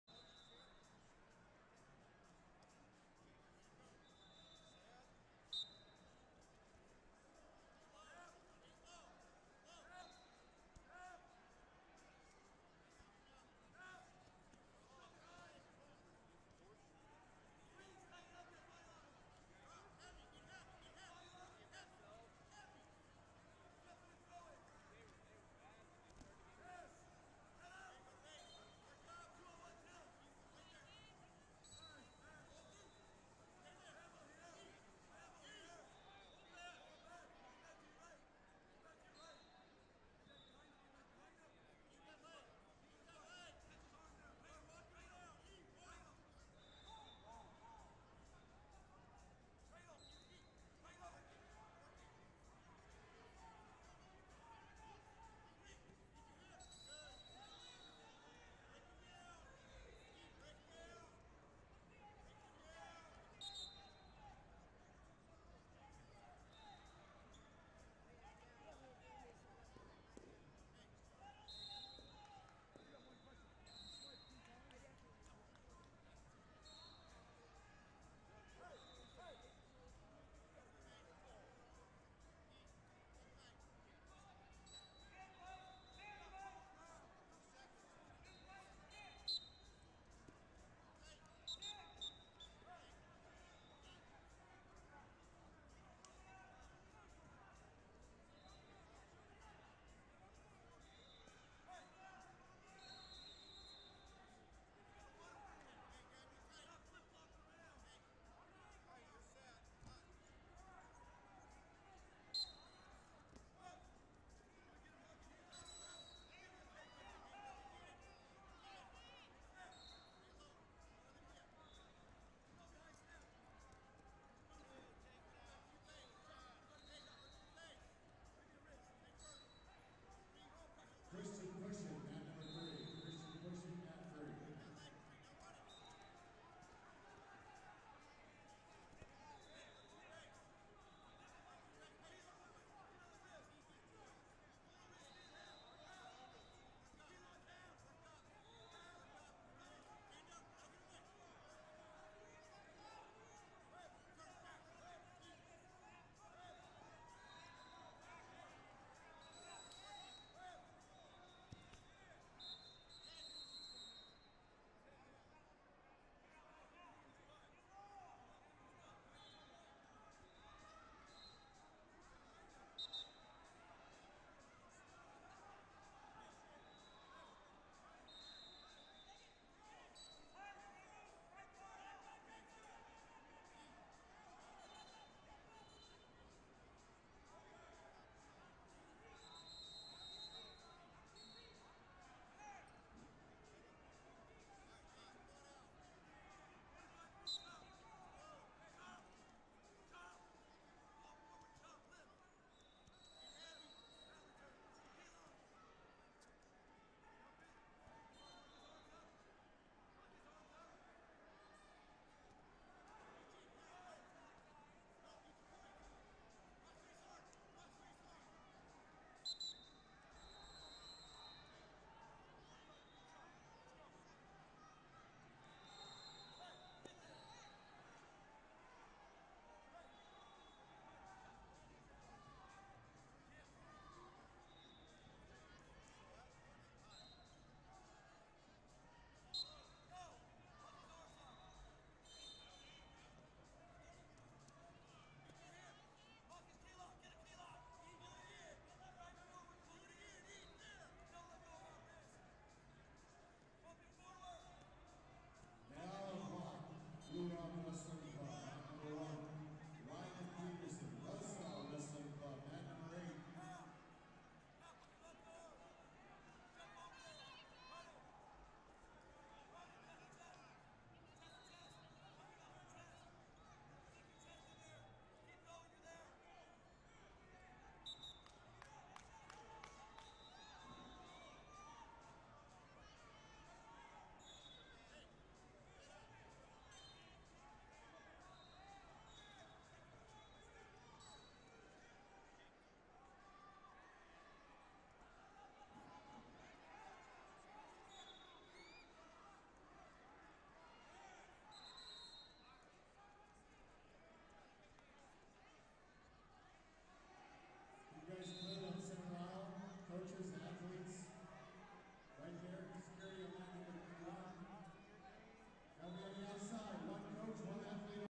108 Lbs Cons. Semi - Isaac Sedeno, Mat Demon WC Vs Easton Bramble, Olympia WC 7bad
FloSports/2021 Folkstyle Tour Of America Northwest Bigfoot Battle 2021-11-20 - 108 Lbs Cons. Semi - Isaac Sedeno, Mat Demon WC Vs Easton Bramble, Olympia WC 7bad